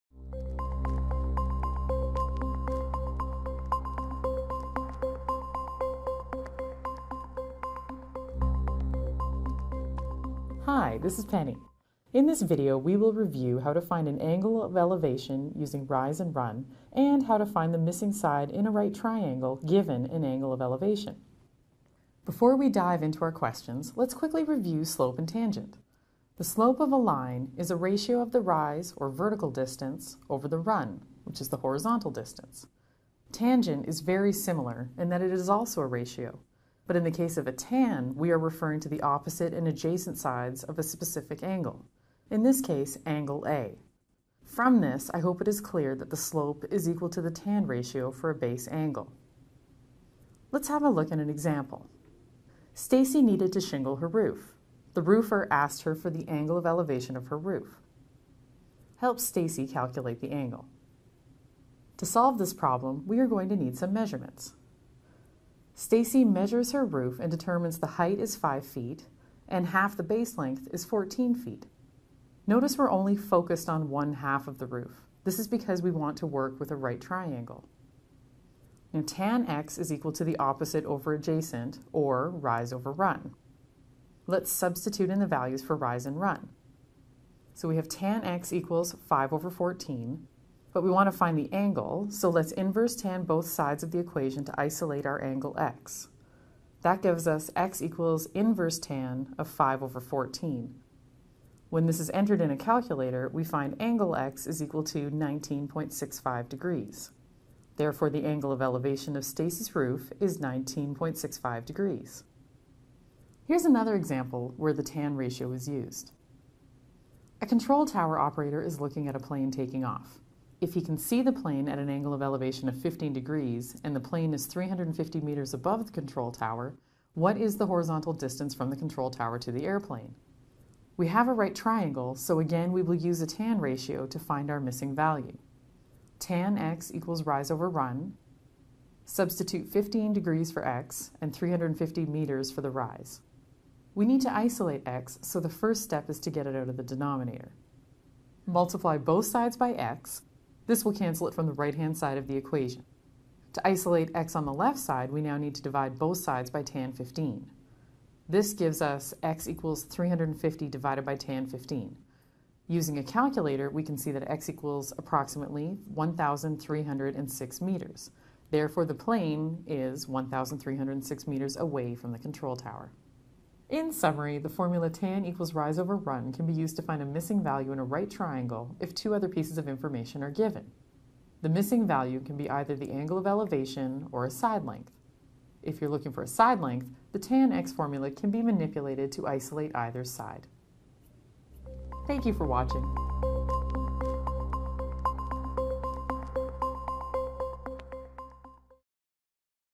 Angle of Elevation and Slope
The video demonstrates how to find angle of elevation  using rise and run. You will learn to address angle of elevation questions.